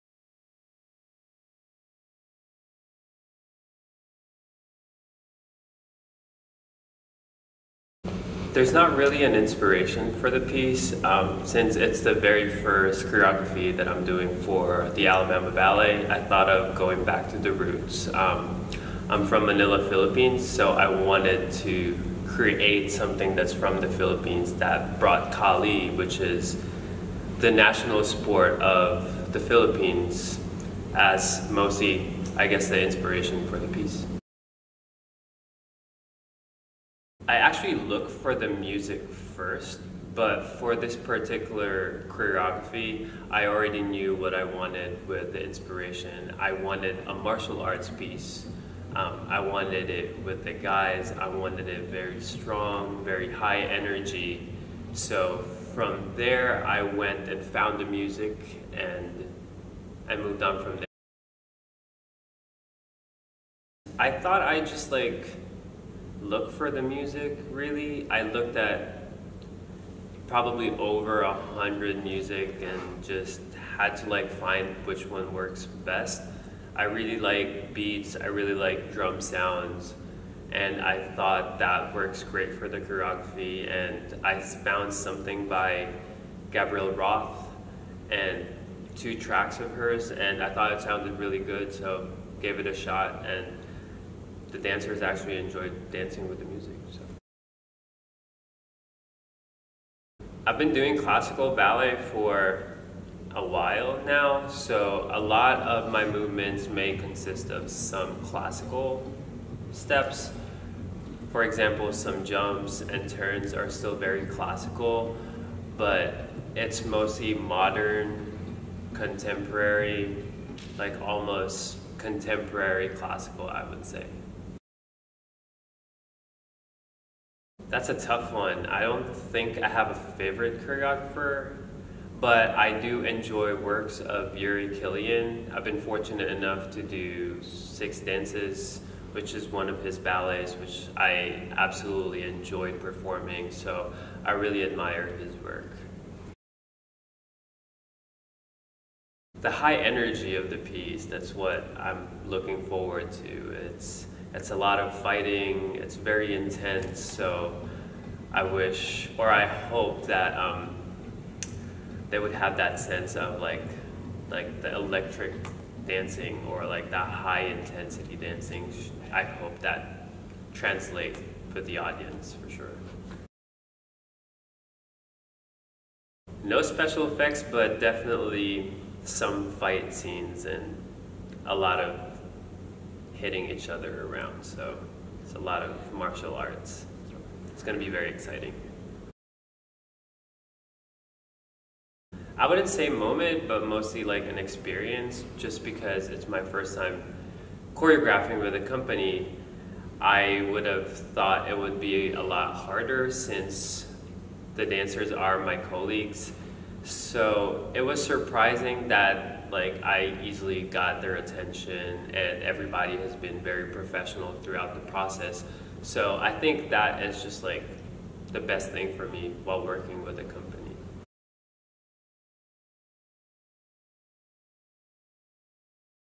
Meet the Choreographers | Frederick Lee Rocas
Learn more about Derek's piece "Kali" and see his piece at Ovation, April 26-28.